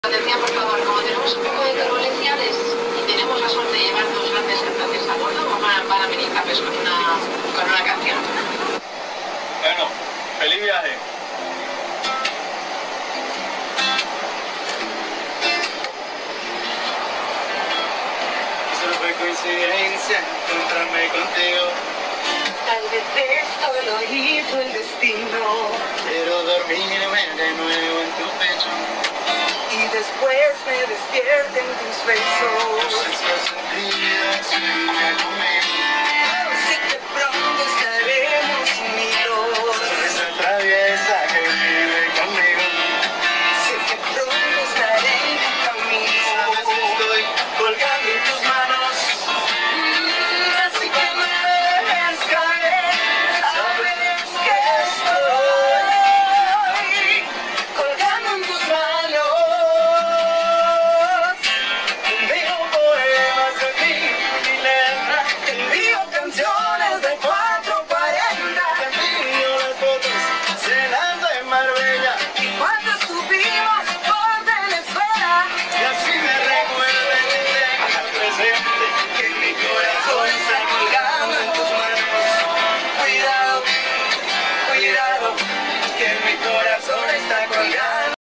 💥En pleno vuelo ,Carlos Baute y Marta Sánchez calman a los pasajeros del avión con su música
martasanchez y carlosbaute calman a los pasajeros de un avión tras sufrir turbulencias con una de sus canciones